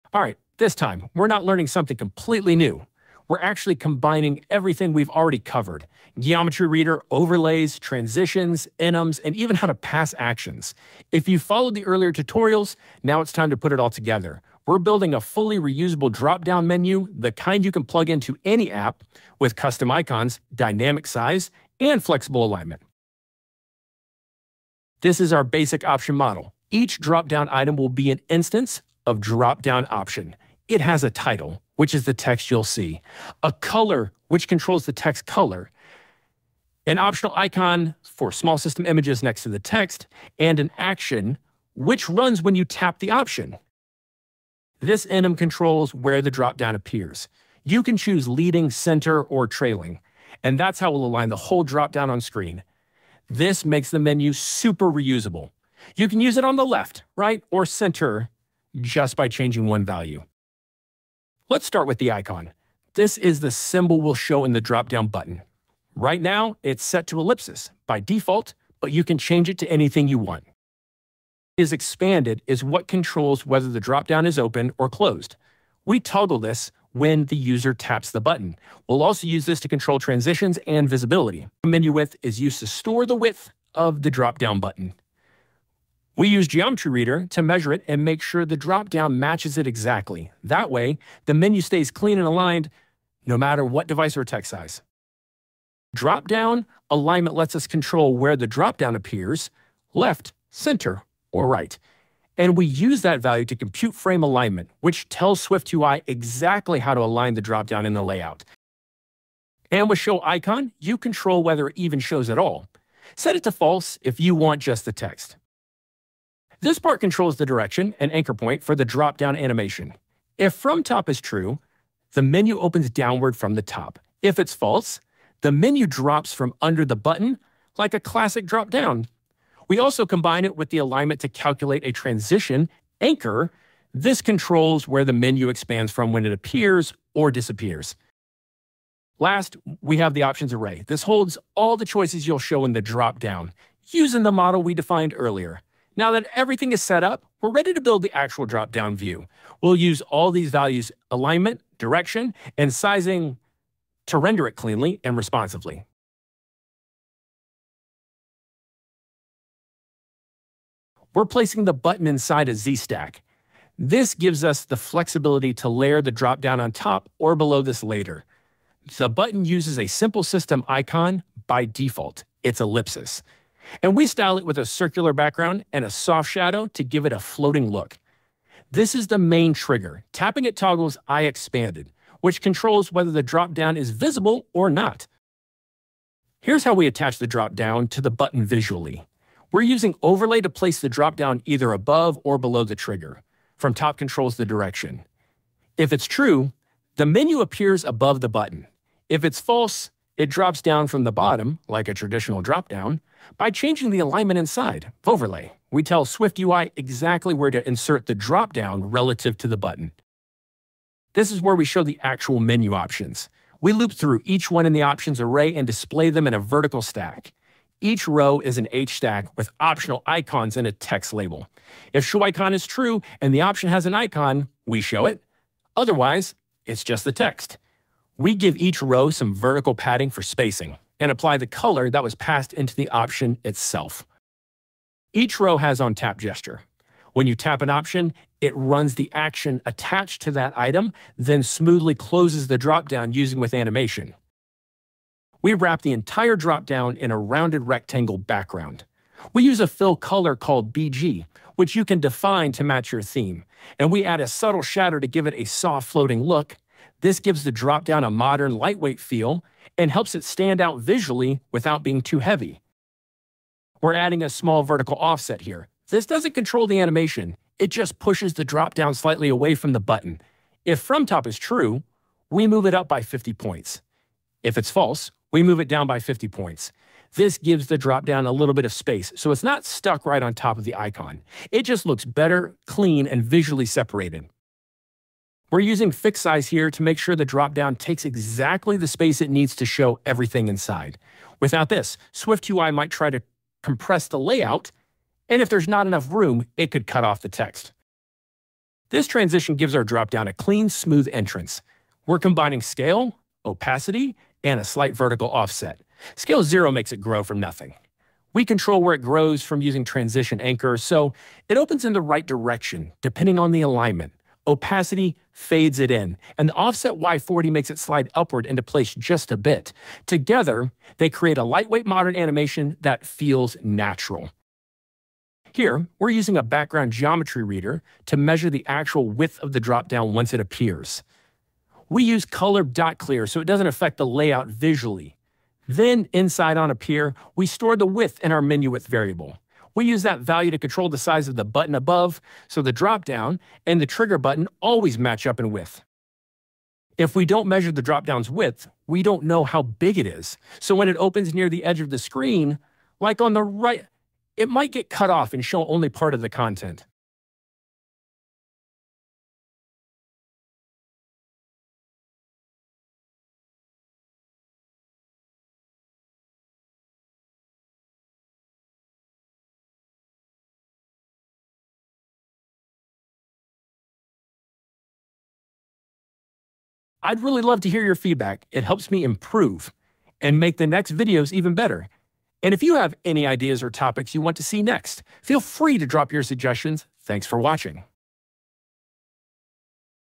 SwiftUI Custom Dropdown Tutorial
In this SwiftUI tutorial, you’ll learn how to build a custom dropdown menu from scratch using GeometryReader, animations, and reusable layout techniques .
We’ll walk through each step in a simple and visual way — from measuring view sizes, controlling alignment, and animating transitions, to building a complete interactive dropdown component .
By the end of this video, you’ll understand how to create a fully dynamic and polished dropdown menu that works on any screen size and adapts to its content automatically .
Perfect for iOS apps that need flexible UI components without relying on UIKit or third-party libraries .
Let’s build it together — clean, reusable, and SwiftUI all the way .